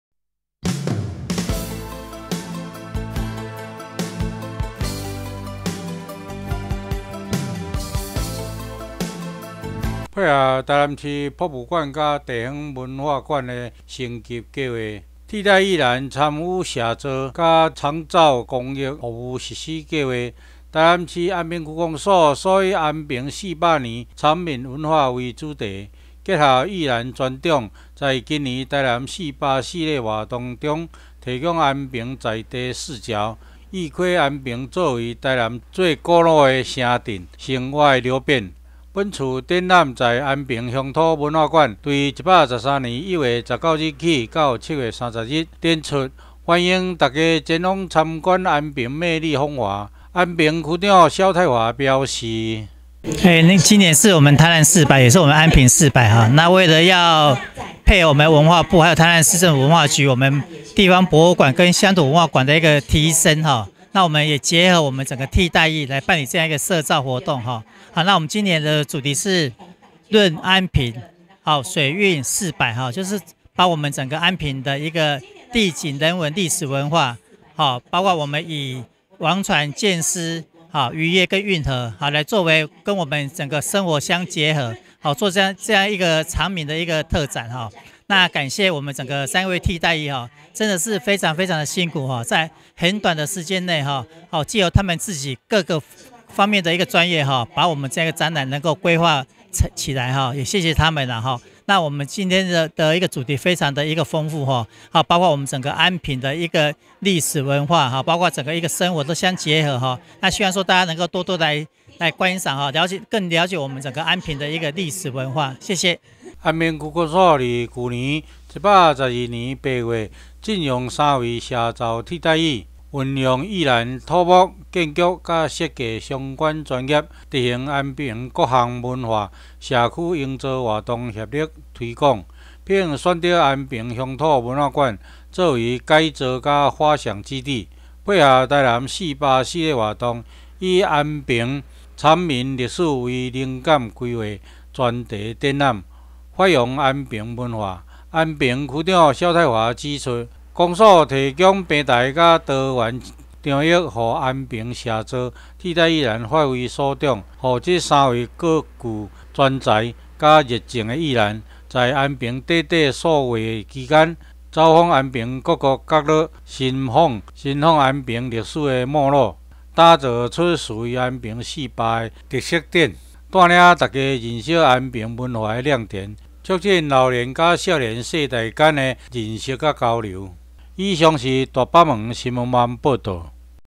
20240119 替代役打造安平鄉土文化館全新展覽 潤安平 水韻四百載常民文化特展—大北門新聞網
配合臺南市博物館及地方文化館升級計畫、替代役男參與社造及長照公益服務實施計畫，臺南市安平區公所以「安平四百年常民文化」為主題，結合役男專長，在今年「臺南400」系列活動中提供安平的在地視角，一窺安平作為台南最古老城鎮的生活流變。本次展覽在安平鄉土文化館從113年1月19日至7月30日展出，歡迎大家前往參觀安平魅力風華。
安平鄉土文化館位於台南市安平區觀音街64號，原為日治時期安平尋常小學校（今西門實驗小學）校長宿舍。曾作為國片《孤味》取景場景之一，也在去(112)年成為實境公益節目《神祕五金行》的拍攝主場景，讓全國觀眾認識安平的特色巷弄及溫暖人情。
    安平區公所於去(112)年8月進用3位社造替代役，運用役男土木、建築及設計相關專業，執行安平區各項文化及社區營造活動的協力推廣，並選擇安平鄉土文化館作為改造及發想基地，配合臺南400系列活動，以安平常民歷史為靈感規劃專題展覽，發揚安平文化。
    本次展覽探討安平自古以來與水之間複雜多面的關係變化。依存、恐懼、或視之為信仰寄託。因此挑選了「劍獅、王船、漁業、運河」四個子題，對主題「潤安平」進行闡述。以安平常民文化的各個面向切入，向觀眾展現安平的文化特色，包含以劍獅和王船為代表的安平傳統民俗，以及安平發展的經濟命脈—漁業與運河。深入探究，會發現這幾項安平傳統文化分別代表了不同的時期，以及人們對於台江水域不同的態度。
市長黃偉哲表示，400年前，臺南是臺灣接觸國際的起點，而安平又是台灣歷史的起源地，除了有2024台灣燈會、國際蘭展、文博會等精彩活動，更多的是內省和深省，回顧過去祖先開拓這門土地，未來要交給未來的下一代子弟，一定會好好的把握向世界向國人向市民展現未來更多的台南400。
民政局局長姜淋煌表示，3位役男們投身區里營造工作，不忘初衷，發揮專業、貢獻社會，是軍旅生涯中的重要篇章，服役期間所營造之公共空間的精彩創作，都是感動人心的偉大力量，再次證明臺南的替代役不僅是最優秀，也是我們一直期待下一代能成長的樣貌。
    安平區長蕭泰華表示，公所提供平台及多元場域讓安平社造替代役發揮所長，讓這3名各具專才與熱情的役男，在安平短短的數月期間，走訪安平各個角落尋訪安平歷史脈絡，打造出屬於安平400的特色展，帶領大家認識安平文化亮點，促進老年與年輕世代間的認識與交流。